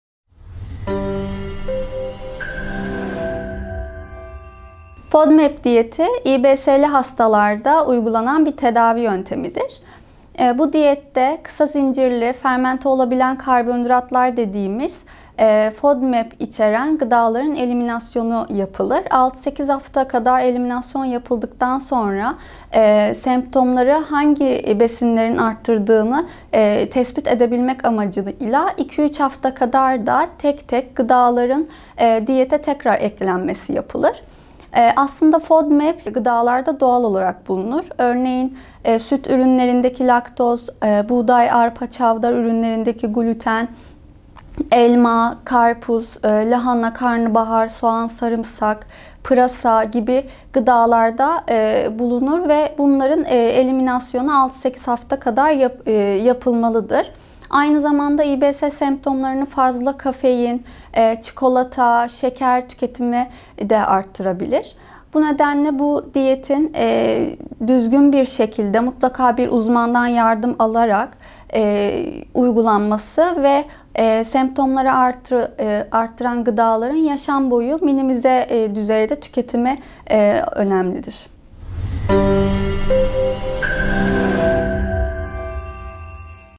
Fodmap diyeti nedir? - Uz. Dyt. N. Sinem Türkmen (Beslenme ve Diyet Uz.)
Memorial Şişli Hastanesi Beslenme ve Diyet Bölümü'nden Uz. Dyt. N. Sinem Türkmen, fodmad diyeti hakkında bilmesi gerekenler hakkında bilgi veriyor.